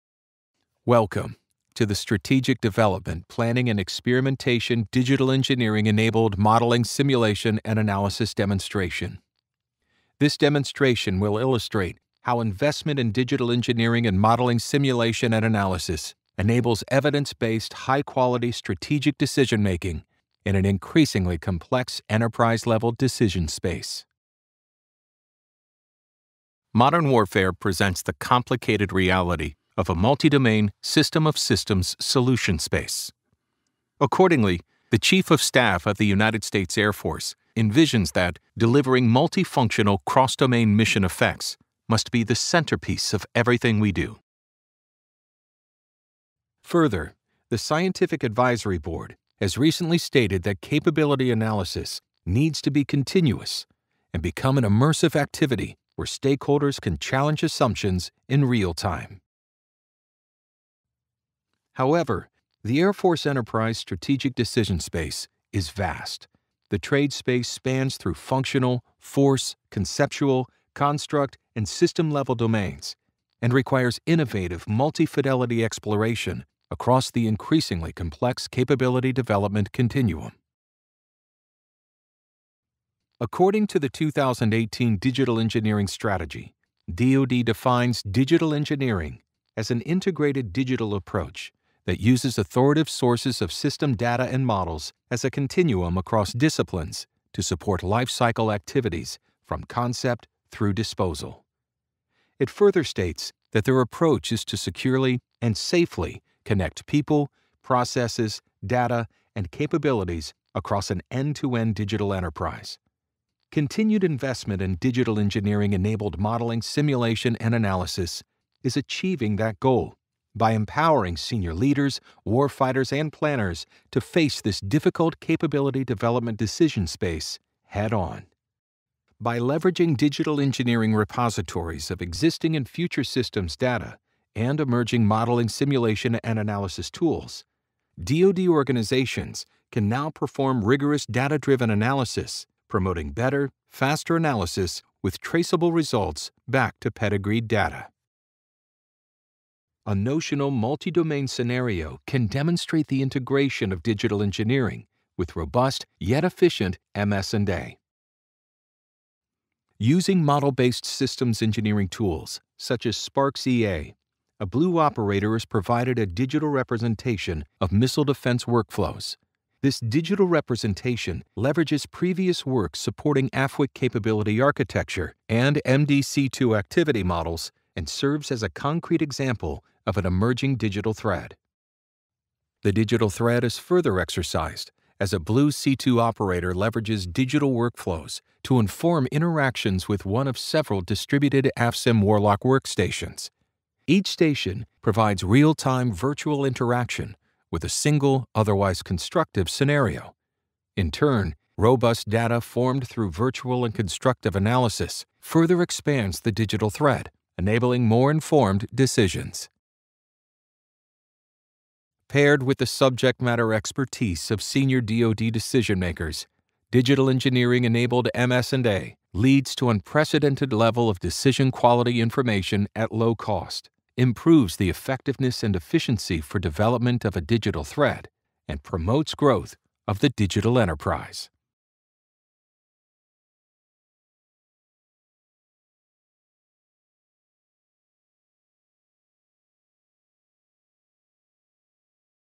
USAF SDPE Digital Engineering-enabled M&S and Analysis demo
Modern warfare presents the complicated reality of a multi-domain system of systems solution space. Accordingly, the Chief of Staff of the USAF envisions that "delivering multi-functional, cross-domain, mission effects must be the centerpiece of everything we do." Further, the Scientific Advisory Board stated that capability analysis needs to be continuous and become an immersive activity, where stakeholders can challenge assumptions in real time.
The Air Force Strategic Development Planning and Experimentation (SDPE) office at Wright-Patterson Air Force Base, Ohio, is responsible for supporting Air Force development planning efforts and conducting experimentation efforts that will result in planning choices for senior Air Force level resource decisions. The office will use live exercises, experiments, war gaming, modeling & simulation, and virtual/hardware prototyping to understand and synthesize future warfighting needs and reconcile those with available and potential capabilities, concepts and emerging technologies.